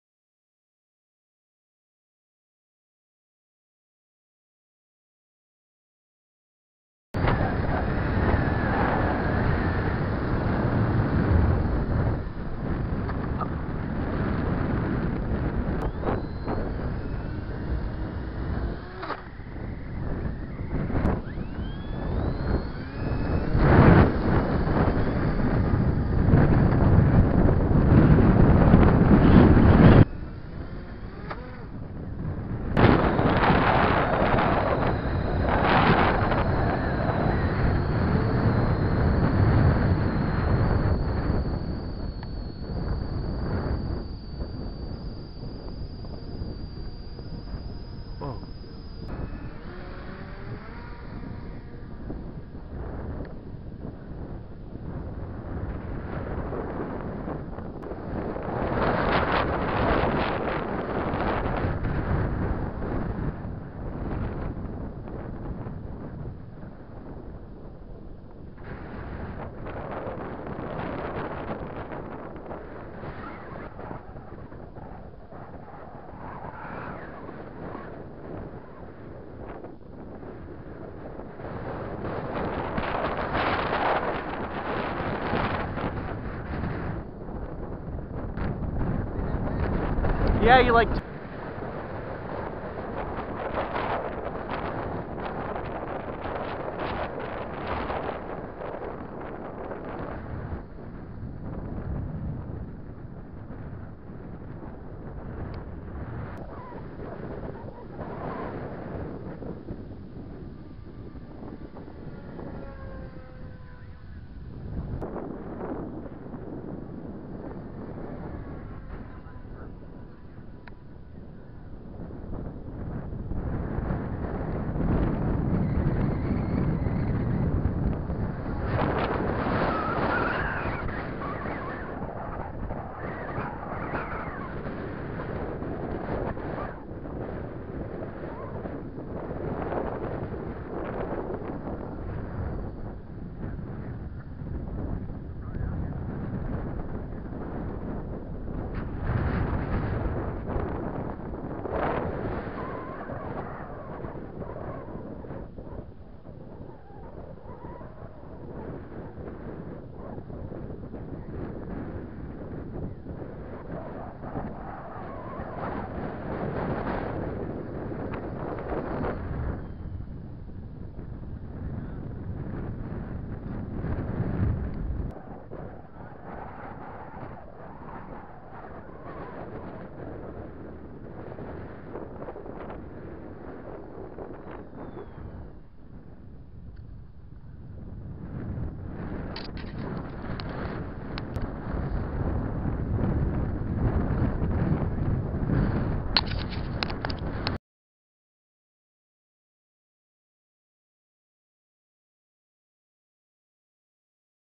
dh 88 landing gear sucks
I enjoy flying it, but taking off and landing sucks.  I think I can fix the landing gear before the next flight to get rid of the problem.